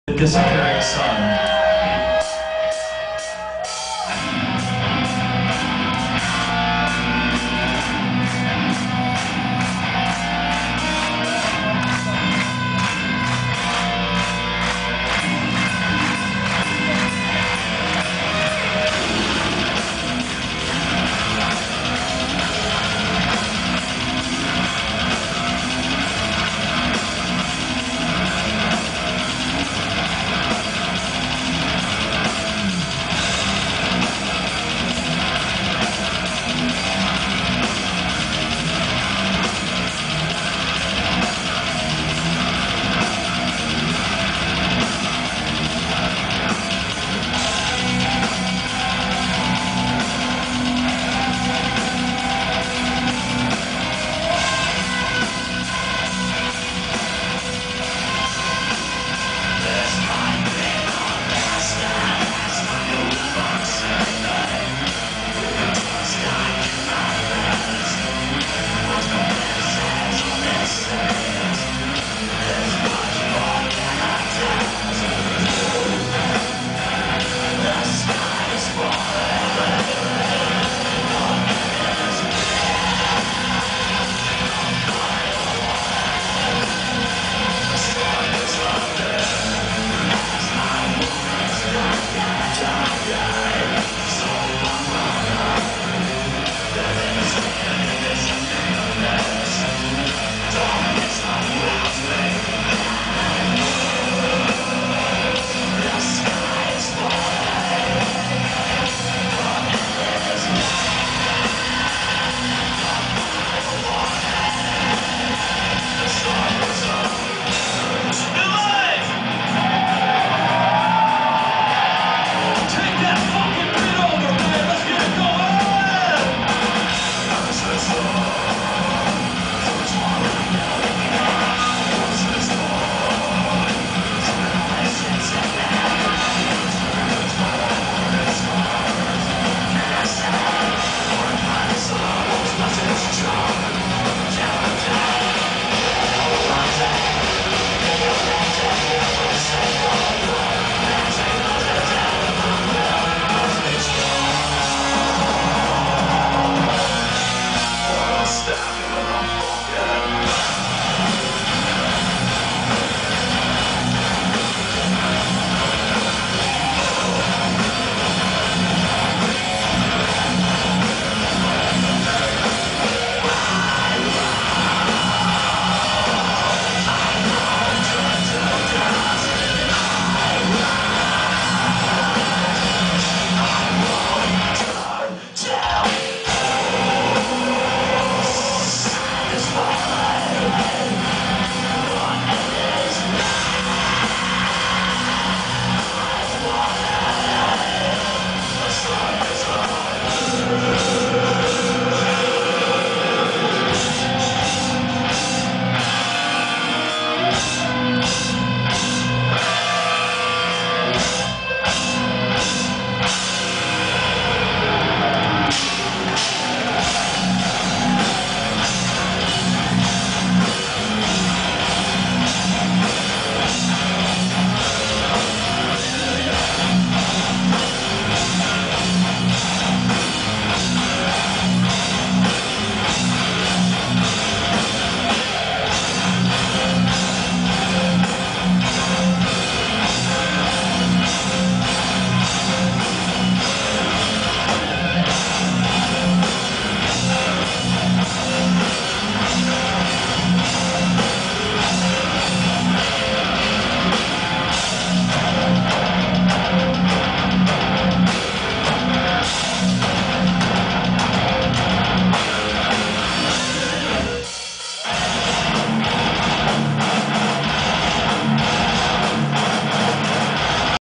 chimaira @The TLA in Philly 11/7/09
chimaira performing at the TLA in Philadelphia, PA on 11/7/09